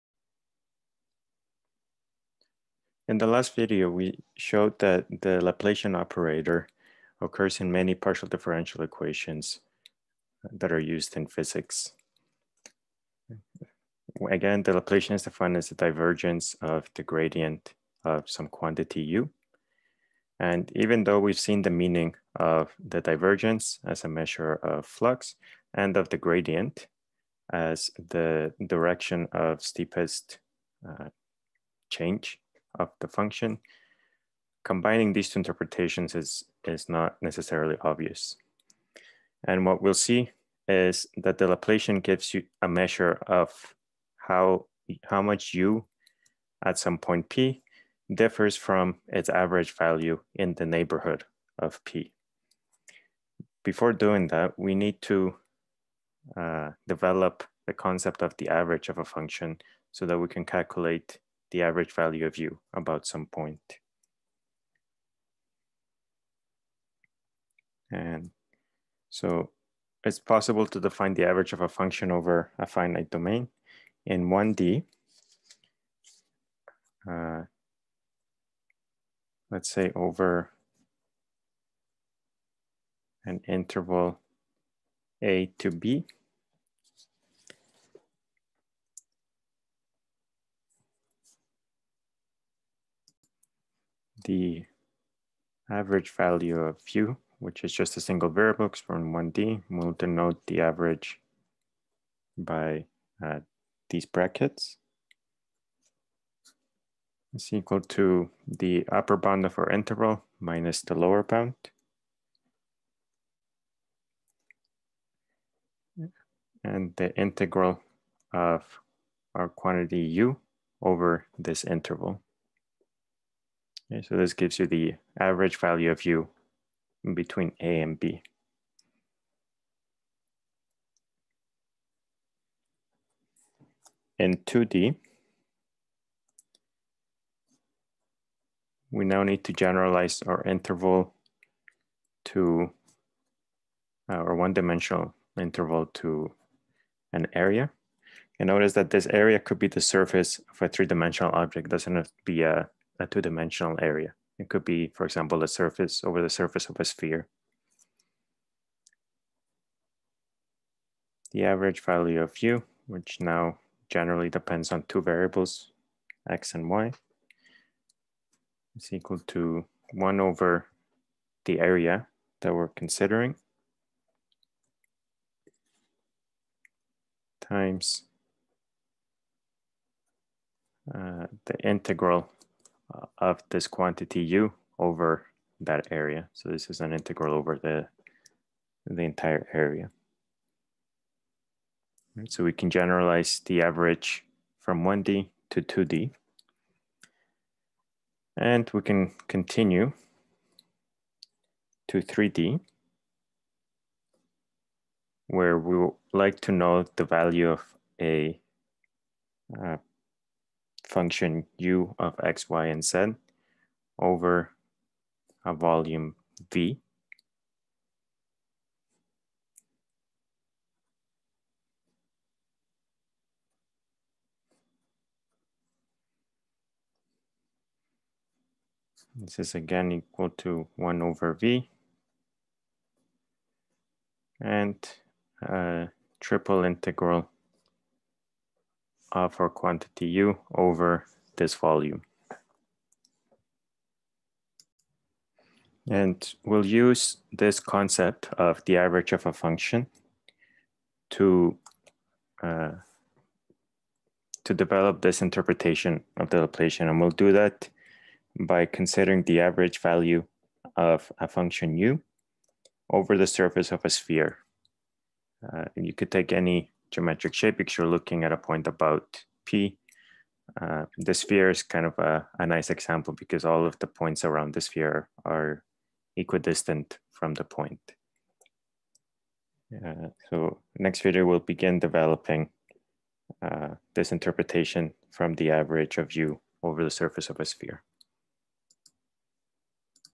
11.2 The average value of a function over a finite domain
Introduces/recalls how to calculate the average value of a function in 1D, 2D and 3D as preparation for discussing the physical meaning of the Laplacian